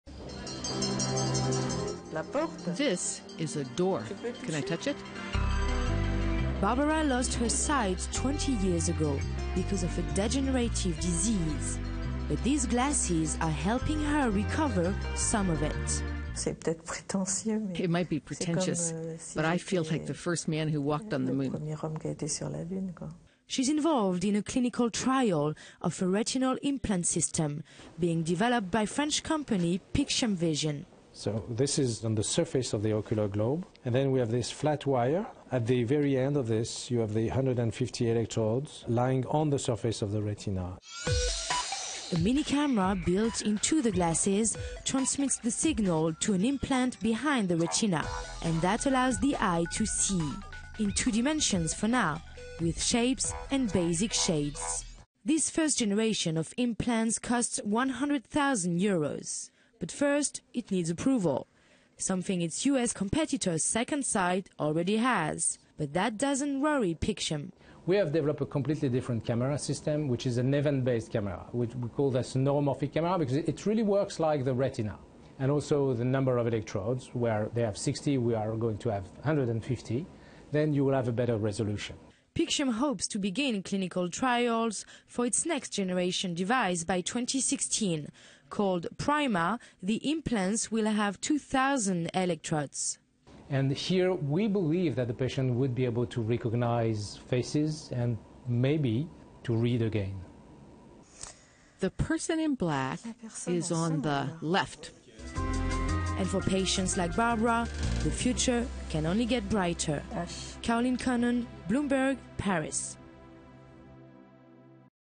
Bionic Eyes: a Look Into the Future
Dec. 5 (Bloomberg) –- What if the blind could see again? Today, more than 40 million people in the world suffer from total blindness. But robotics and scientific research could change that, with European and U.S. companies fighting for the market. Bloomberg’s Caroline Connan met with the French company developing the next bionic eye. (Source: Bloomberg)